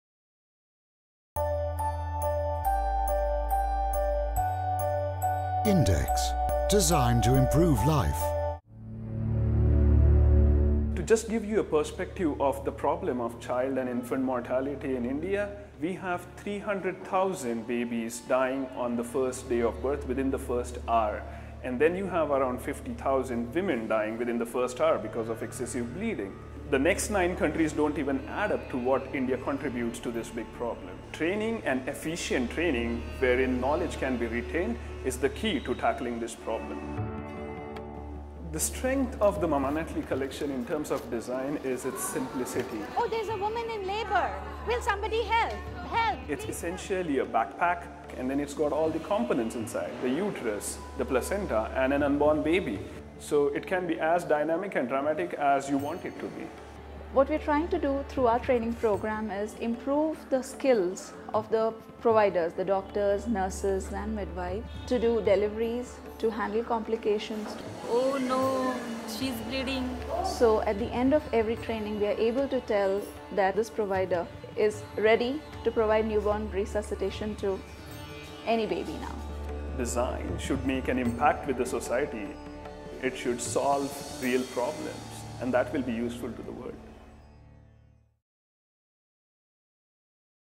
The Natalie Collection, INDEX: AWARD 2013 FINALIST
Two of the UN Millennium Development Goals are to reduce child and maternal mortality by 2015. Norwegian product developers have designed a trio of highly effective and low cost solutions to do just that. The Natalie Collection consists of the NeoNatalie Suction, NeoNatalie Newborn Simulator and the MamaNatalie Birthing Simulator and are all designed to save thousands of lives. Vote now